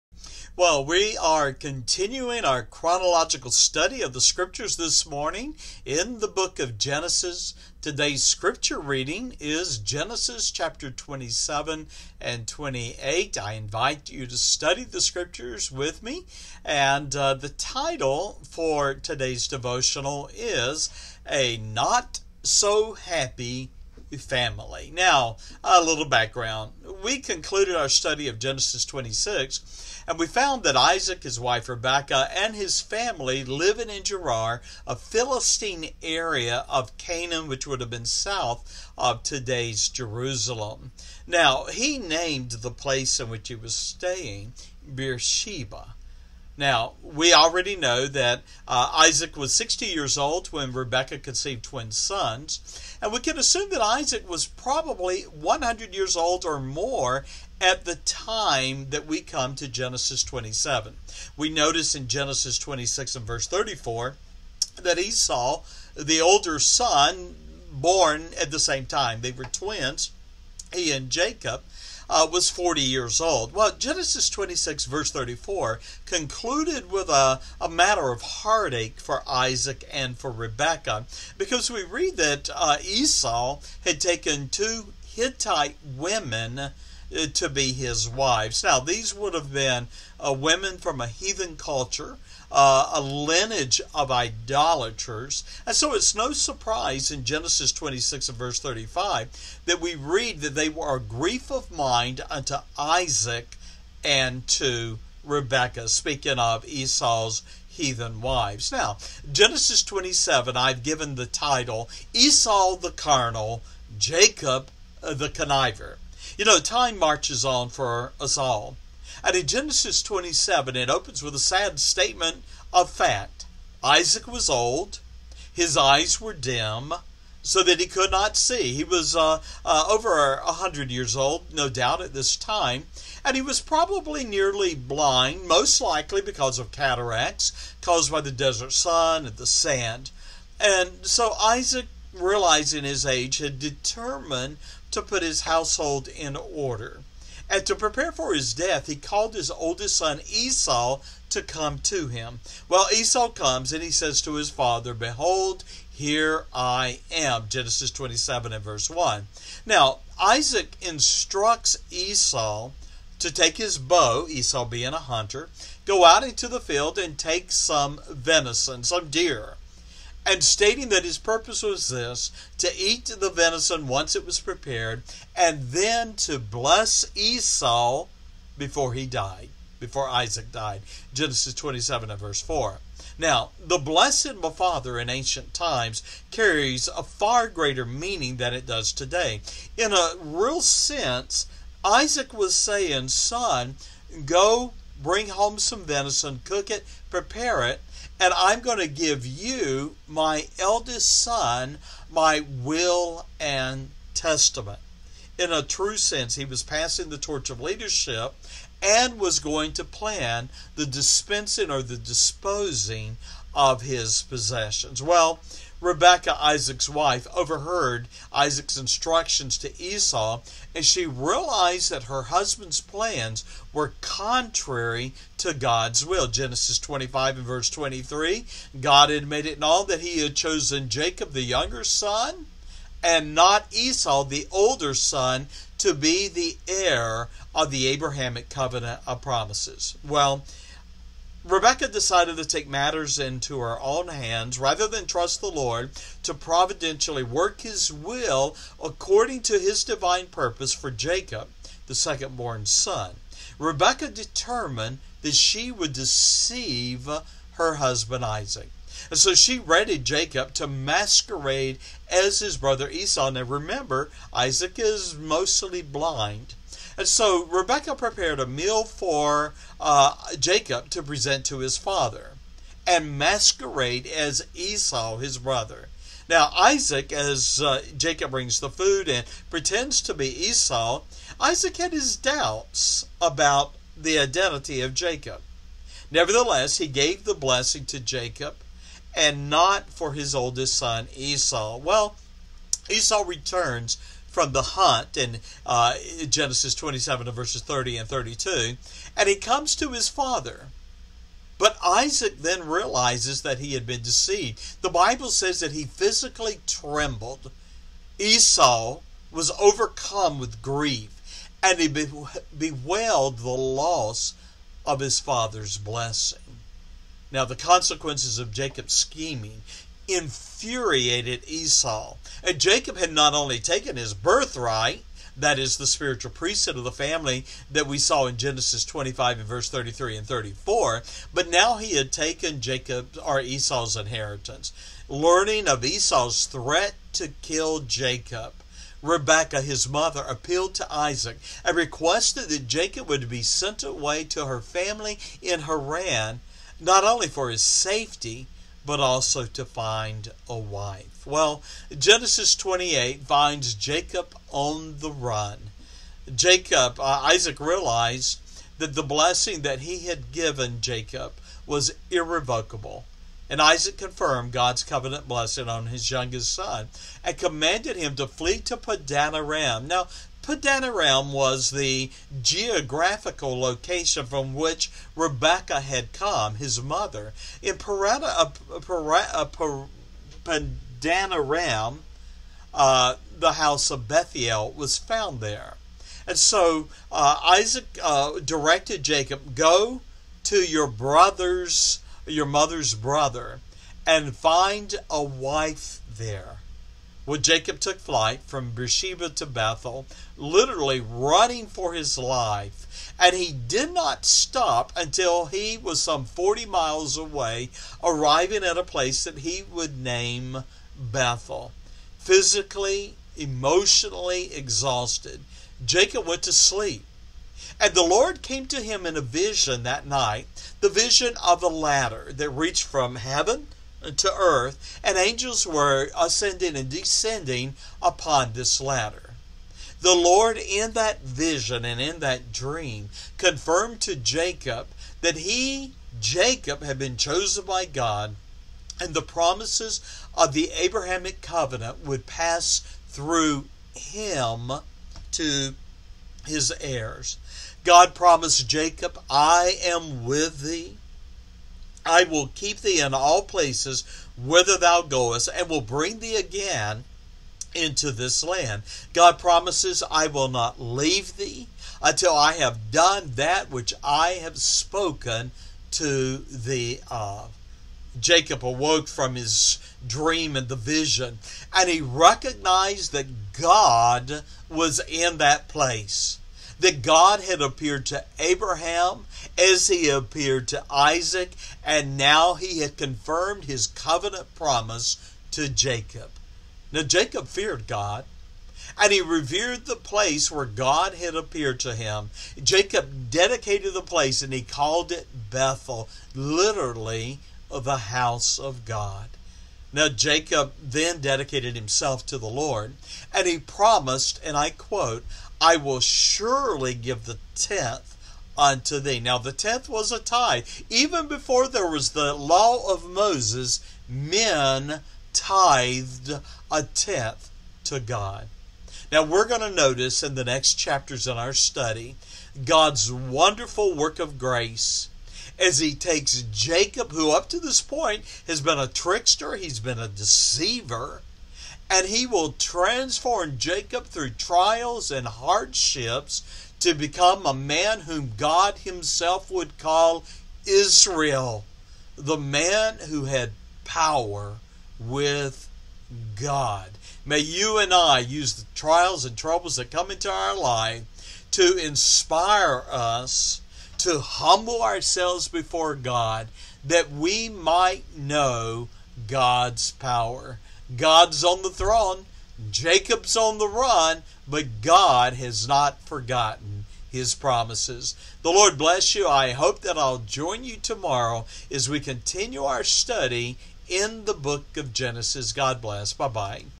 A Not So Happy Family (Genesis 27-28)
We continue our chronological video devotional with today's Scripture, Genesis 27-28.
We will notice today the fulfillment of the prophecy that the sons of Isaac and Rebekah would be like two nations at war.
In Genesis 27 we will consider, “Esau the Carnal, and Jacob the Conniver.” In Genesis 28 we find Jacob, "On the Run, and Alone: When God Speaks—Listen!"